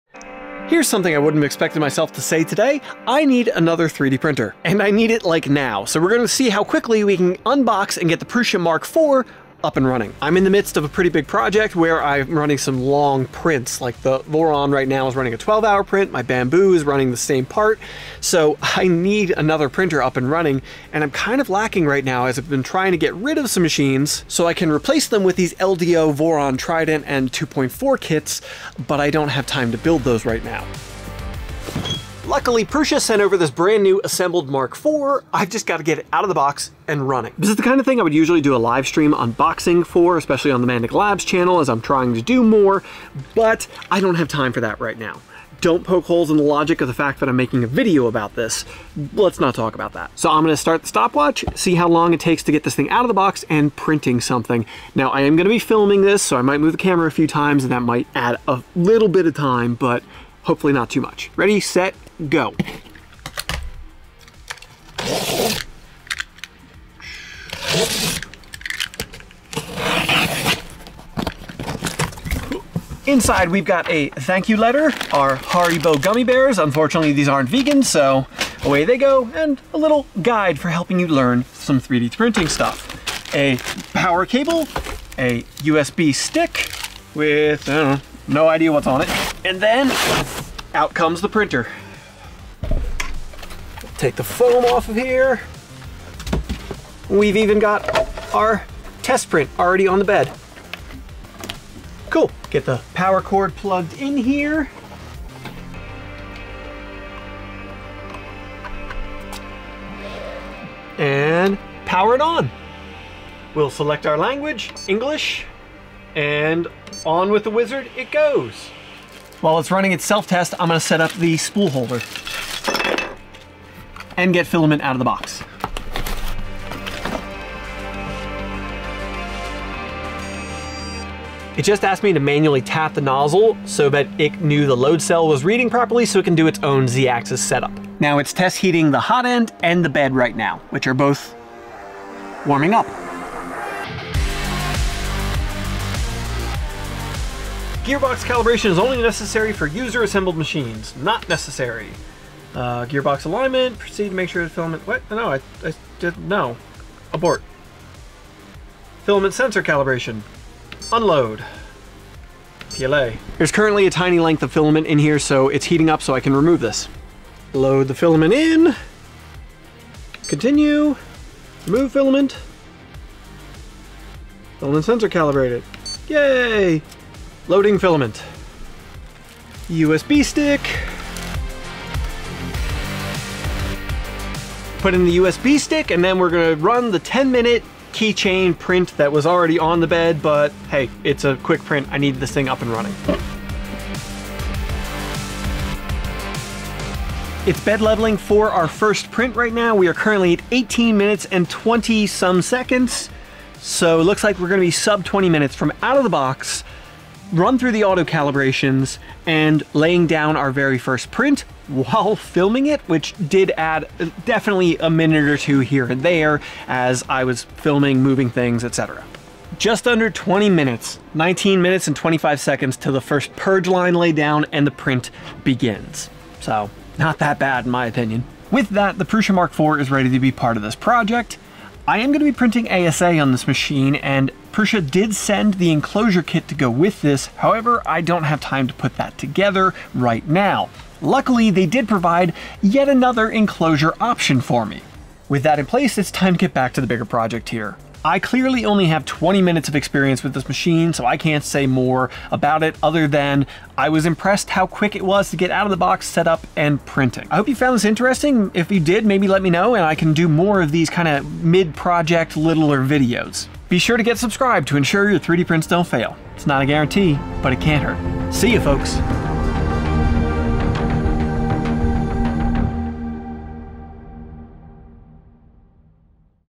Unboxing Prusa Mk4 & Printing in Record Time
My studio needs another printer, yes really. The folks at @Prusa3D sent over the Mk4 so let's see how fast we can get it Unboxed & Printing! From removing the assembled Prusa Mk4 from the box to running a test print, how long will it take? Along with a bonus budget 3d printer enclosure setup!

Music from BackingTrack.gg